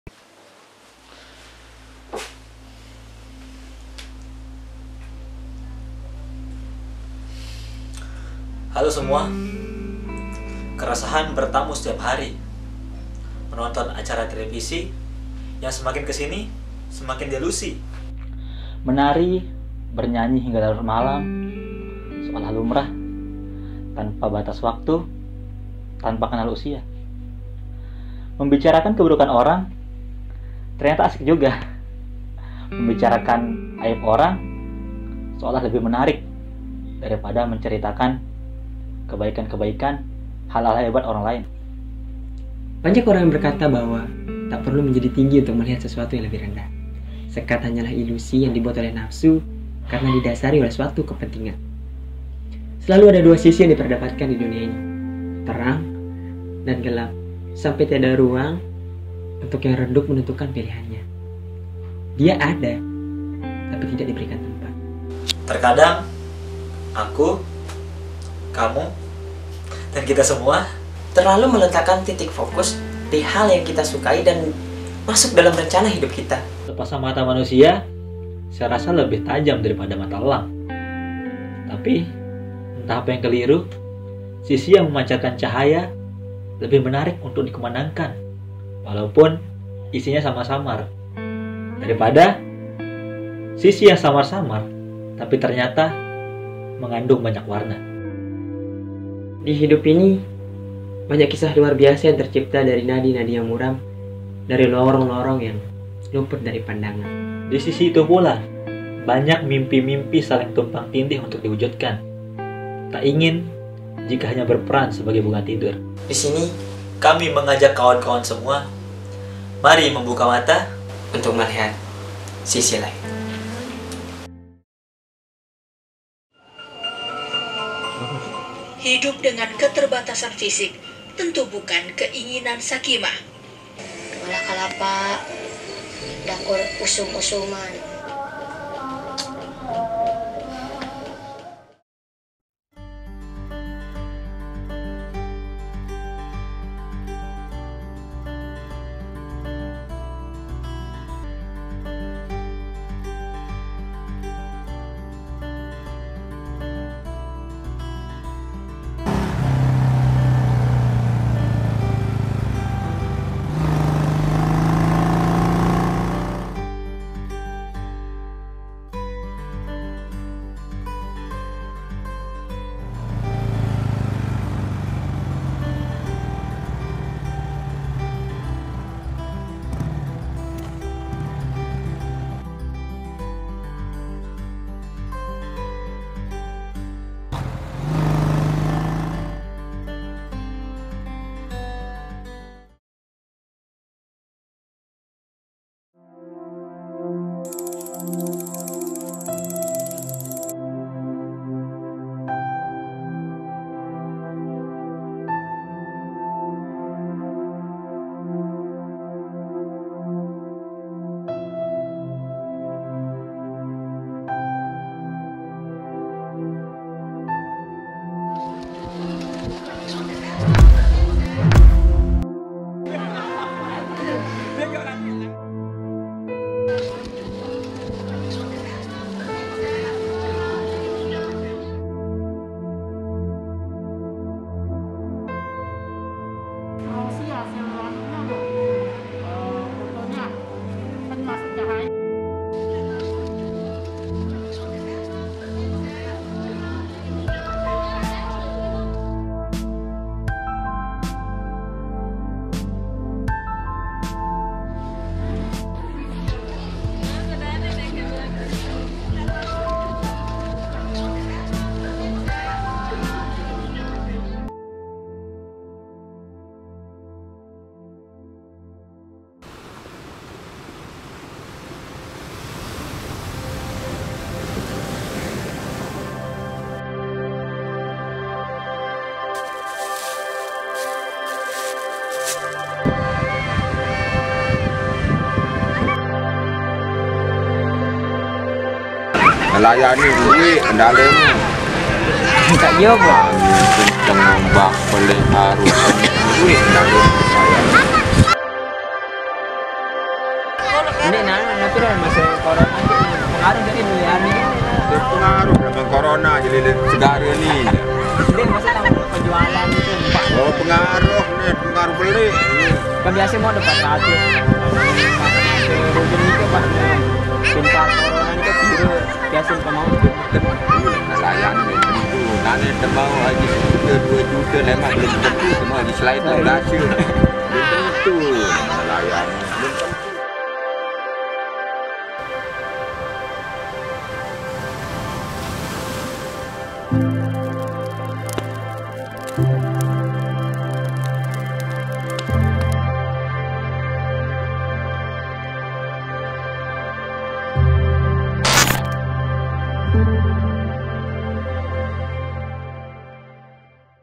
ADA SESUATU DI SISI LAIN (INTRODUCTION)
Ada hal samar-samar yang tertinggal di sudut-sudut yang renyah akan tawa.
Banyak keresahan di luar jendela yang kita buka dan banyak pula yang menutup mata.
Selalu ada dua hal yang berseberangan. Di antara dua, ada satu yang menonjol.
Oleh karena itu, kami mengajak kawan-kawan semua untuk melihat persprektif Sisi Lain.